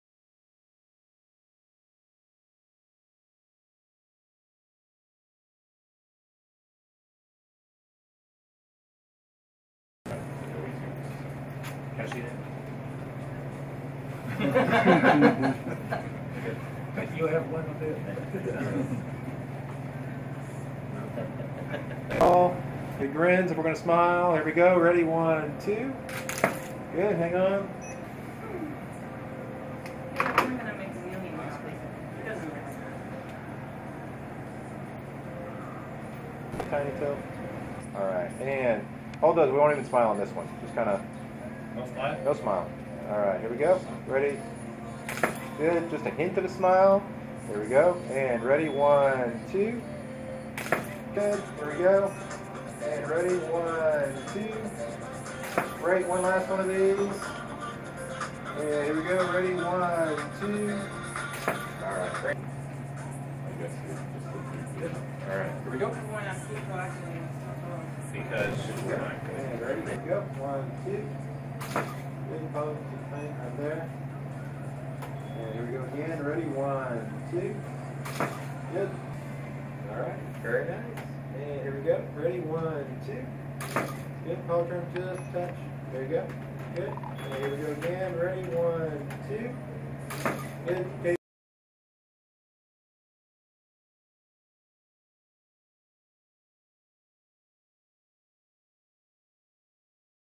Servizio fotografico sull'equipaggio della Expedition 26/27
L'equipaggio della Expedition 26/27, fra cui l'astronauta ESA Paolo Nespoli, posa per un servizio fotografico ufficiale.

Credit: NASA